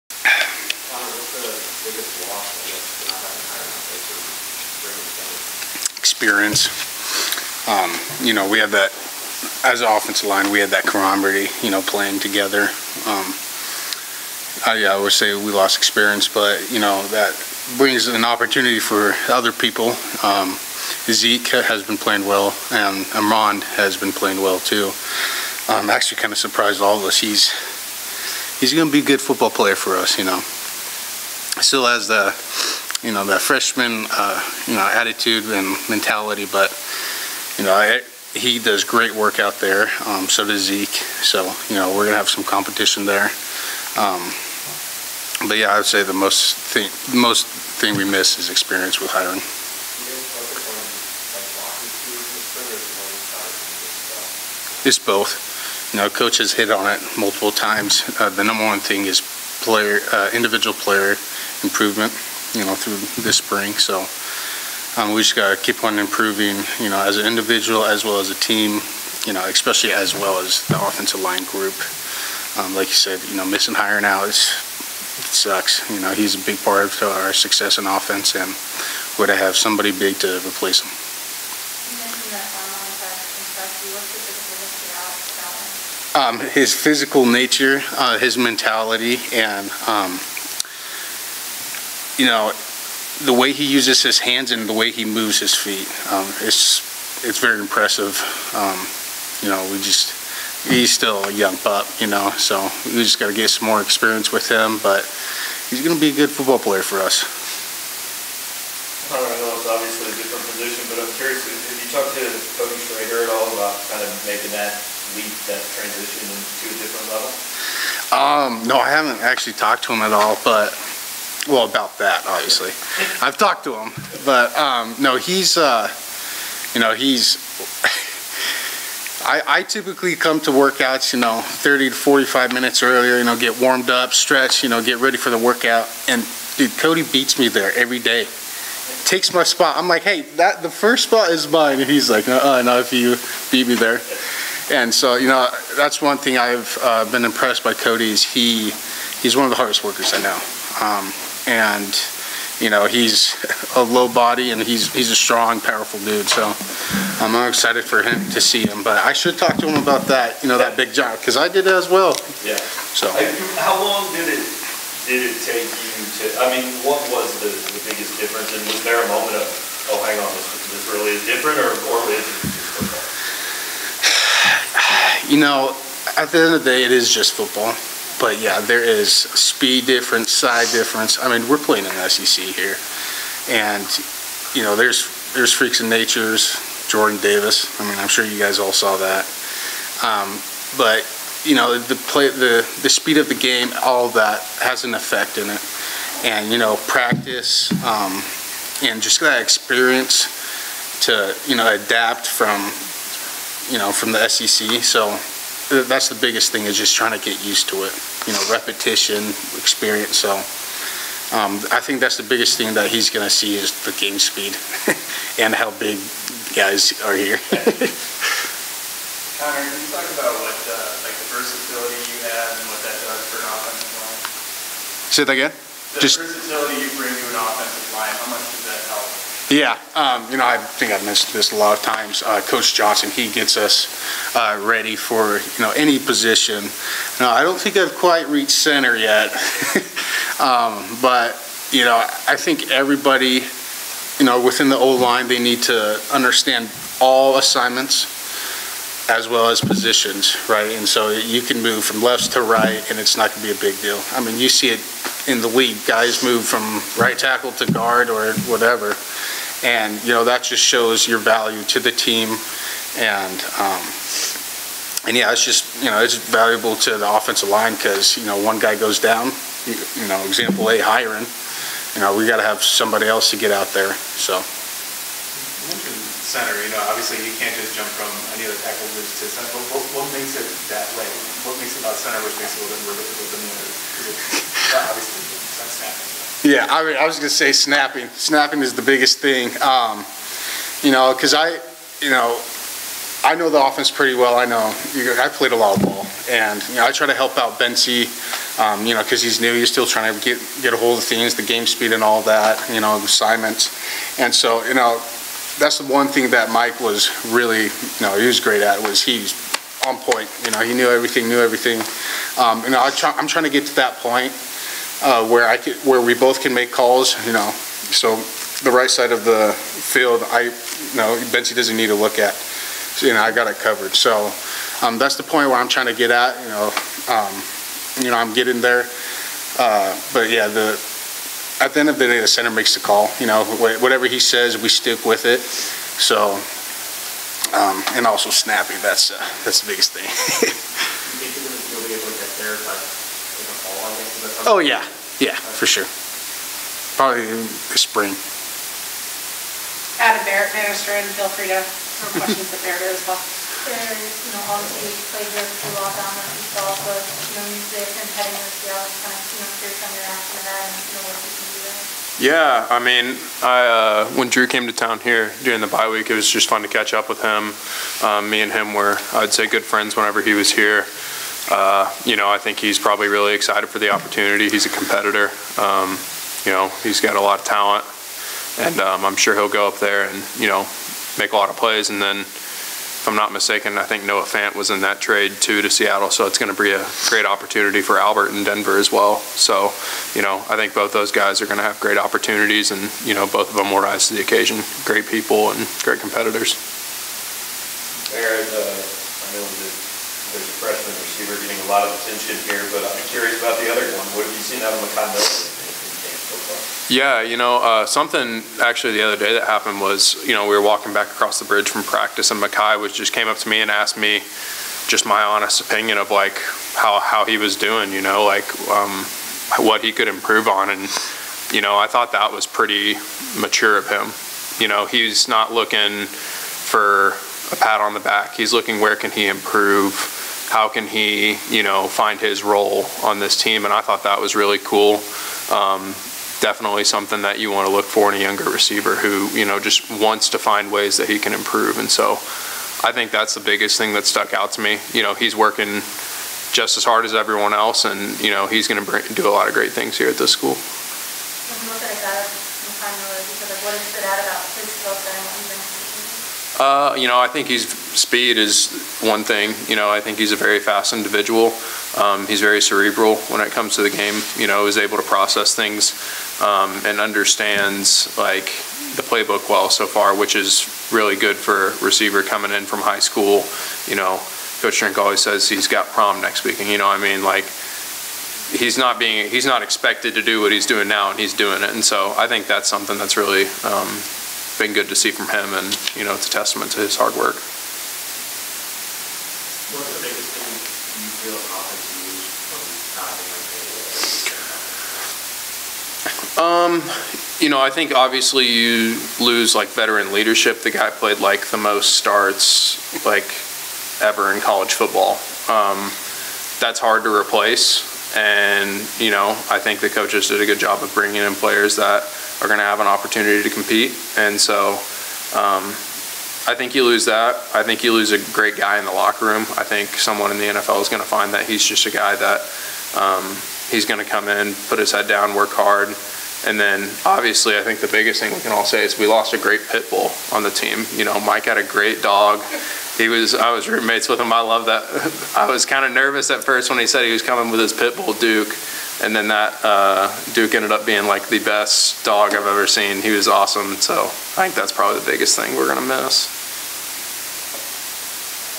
Full interview with Mizzou OL Connor Wood and WR Barrett Banister after spring practice ...
Full interview with Mizzou OL Connor Wood and WR Barrett Banister after spring practice 030822